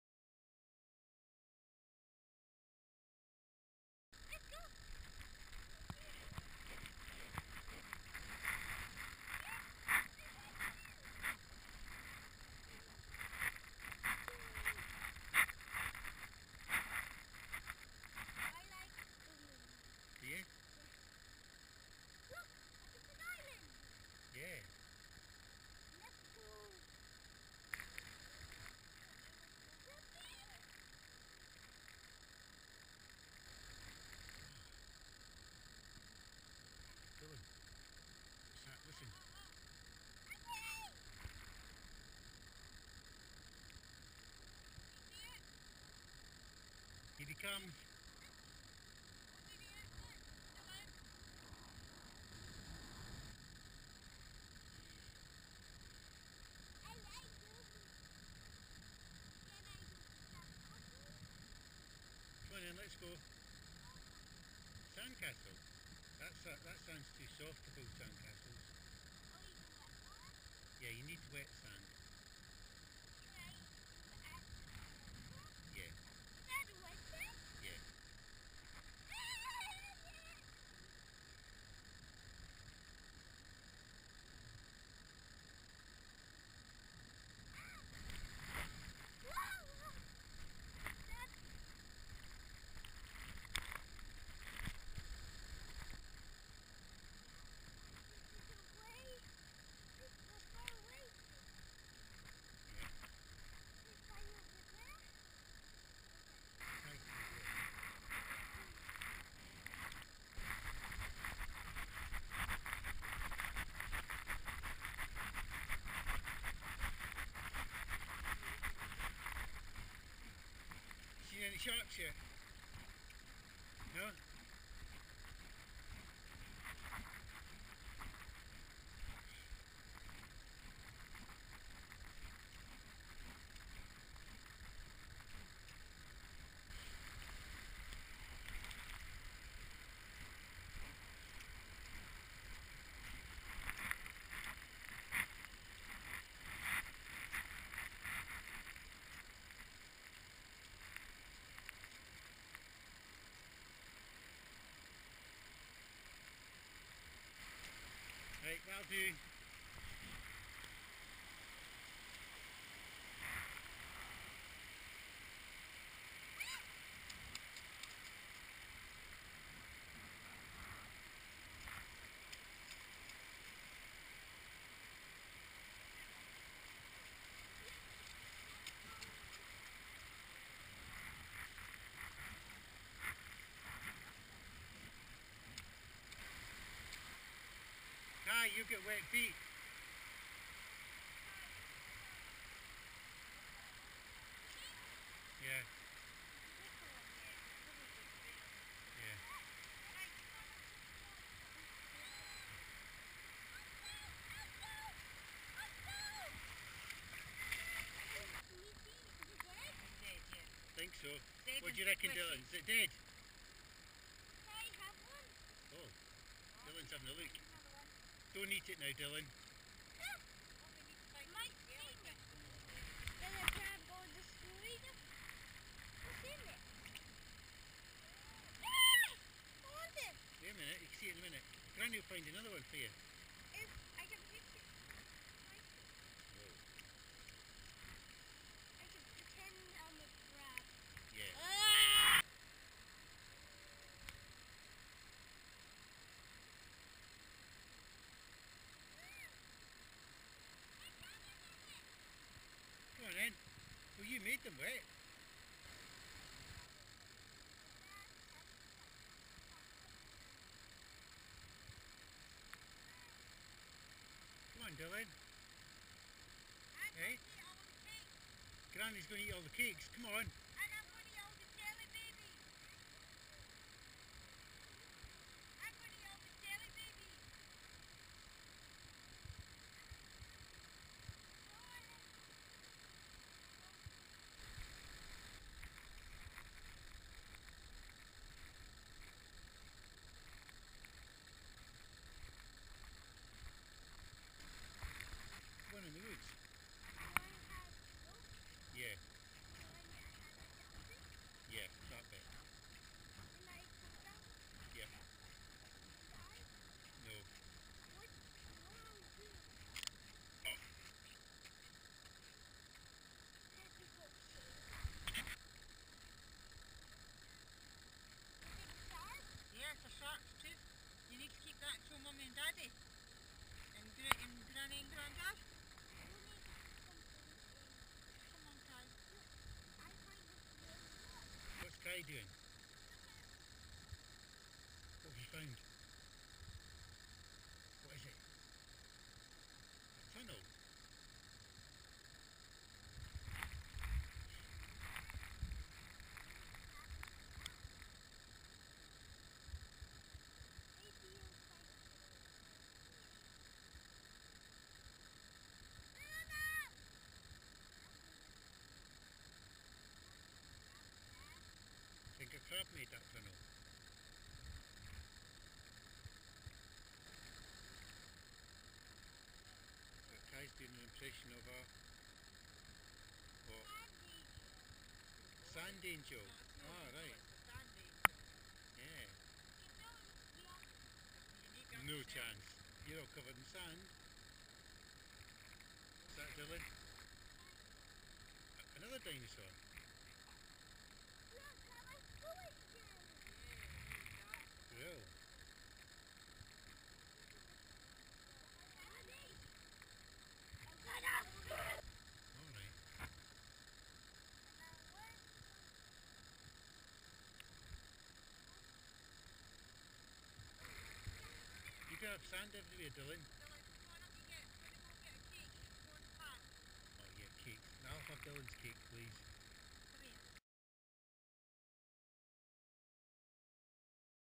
Tentsmuir beach
Kai and Dylan at tentsmuir beach